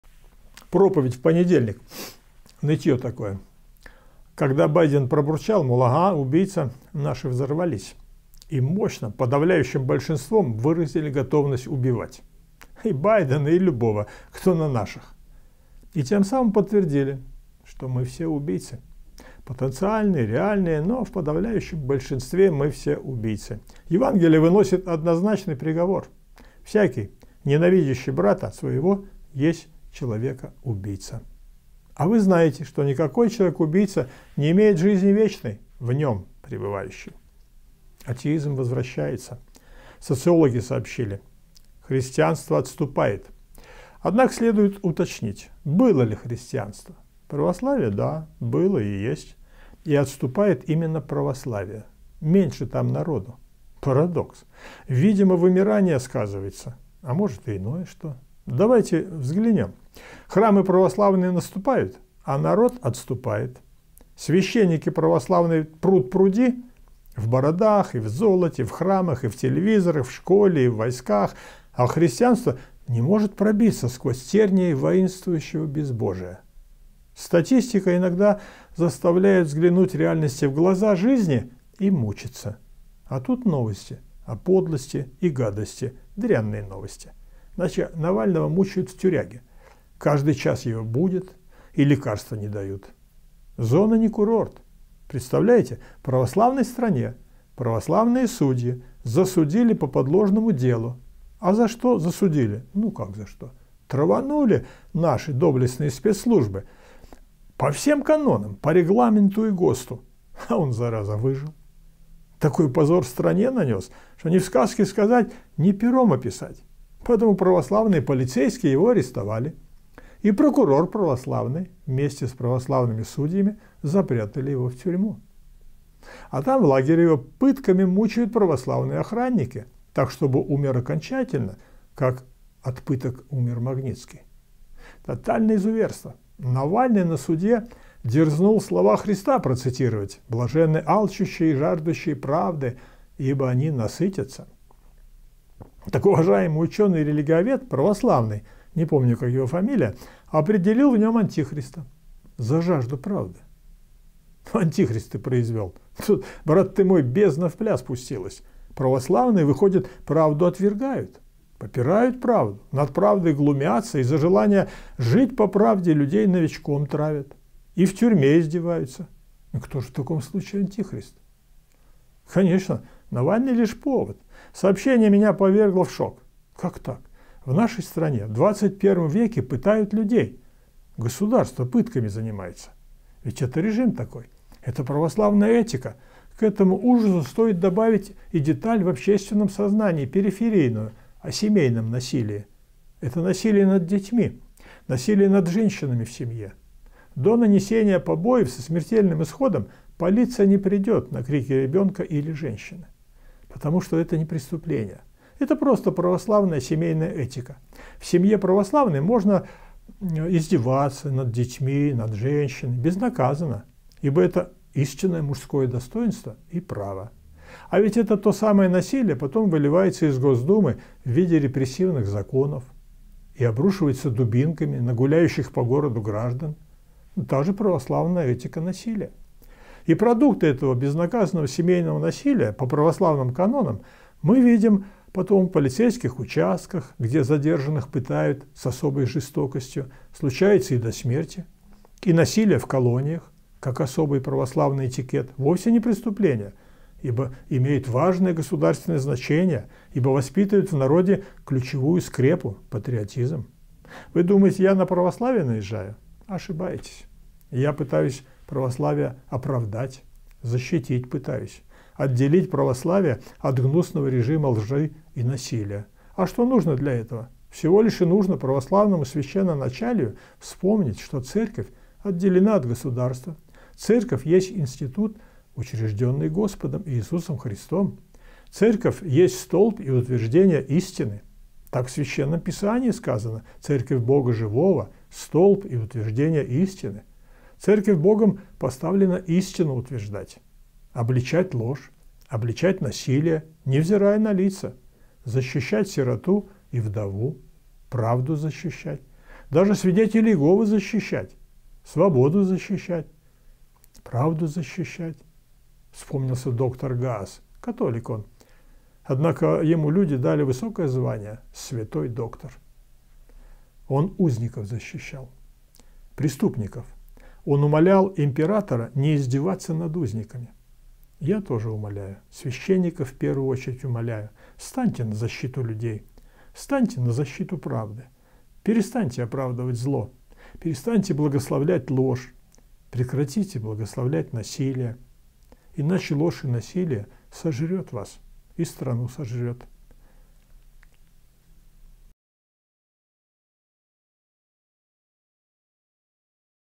Чистый четверг с Юрием Сипко #12 | не благословляйте ложь и насилие
Проект чтений Богом вдохновленных текстов "Чистый четверг" с братом во Христе Юрием Сипко сегодня посвящен теме: насилия и лжи. Канал "Взгляд с Небесной" предоставляет это видео на суд наших уважаемых зрителей. Текст читает автор.

Каждую неделю мы выпускаем 3 новых интересных видео.
Вы можете поддержать нас финансово:
YouTube канал ВЗГЛЯД С НЕБЕСНОЙ
Слово Жизни